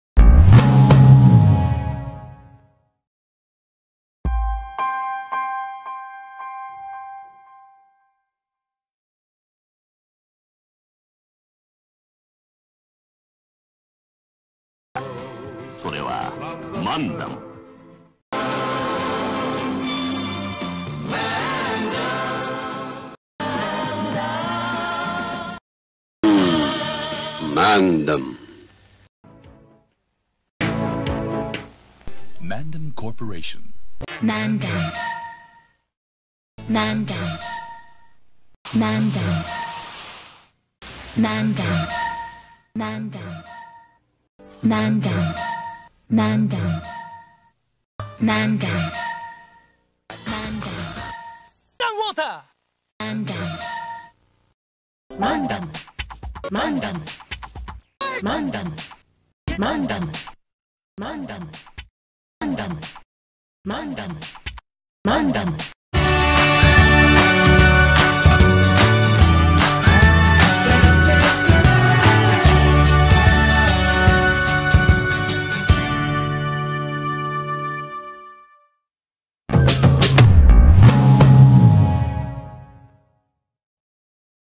Mandom Logo History | Evologo [Evolution of Logo]
Watch Mandom Logo History now on Evologo, Evolution of Logo by McRizzwan!

Watch Evologo, the Evolution of Logo, every Thursdays @10.00am, Saturdays @10.00am and Sundays @04.00am (UTC), on McRizzwan!

Credit to these people who have found these logo finds, which helped me make this video.